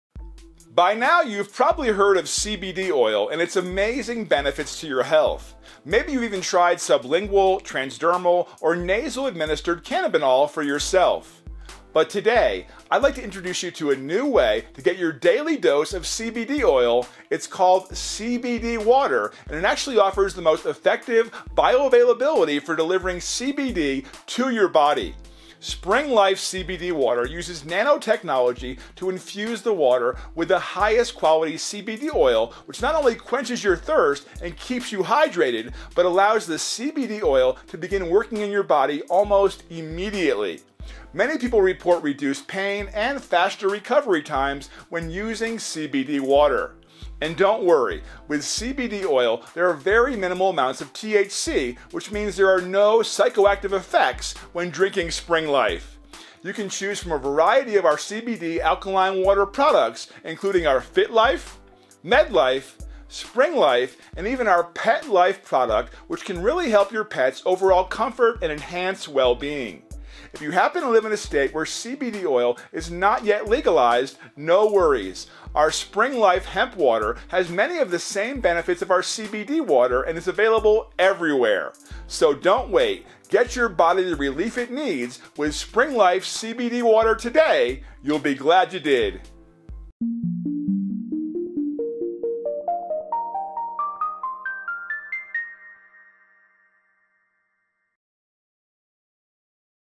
Spring Life CBD Water with the highest quality CBD oil
By now you have probably heard of CBD oil and it’s amazing benefits to your health, maybe you have even tried sublingual, transdermal or nasal administered cannabinol for yourself.

But today, I’d like to introduce you to a new way to get your daily dose of CBD oil – it’s called CBD water and it is actually offers the most effective bioavailability for delivering CBD to your body.

Spring Life CBD water uses nanotechnology to infuse the water with the highest quality CBD oil – which not only quenches your thirst and keeps you hydrated -- but allows the CBD oil to begin working in your body almost immediately!

Many people report reduced pain and faster recovery times when using CBD water.  And don’t worry, with CBD oil there is very minimal amounts of THC – which means there are no psychoactive effects when drinking Spring Life!

You can choose from a variety of our CBD alkaline water products, including our Fit Life, Med Life, Spring Life and even our Pet Life product – which can really help your pet’s overall comfort and enhanced wellbeing!

If you happen to live in a state where CBD oil is not yet legalized – no worries – our Spring Life Hemp water has many of the same benefits of our CBD water and is available everywhere!

So don’t wait – get your body the relief it needs with Spring Life CBD water today!  You will be glad you did!